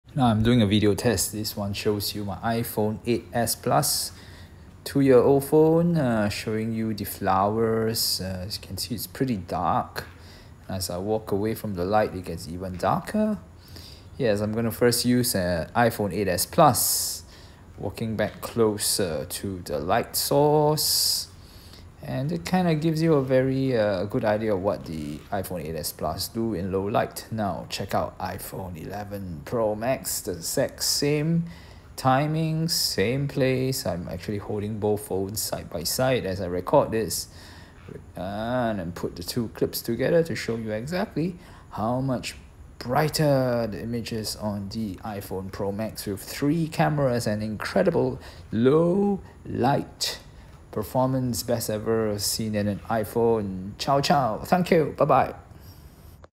iPhone 11 Pro Max versus iPhone 8S Plus Low Light Video Comparison
Taking out my iPhone 11 Pro Max and my iPhone 8S Plus for a low light video comparison.  Did a video of the same flowers, in low light.  Took the video at the same time.  Just look at how much brighter the iPhone 11 Pro Max results are.  It is simply amazing, how much light the new iPhone 11 Pro Max can capture using the 3 camera system.  Cool stuff!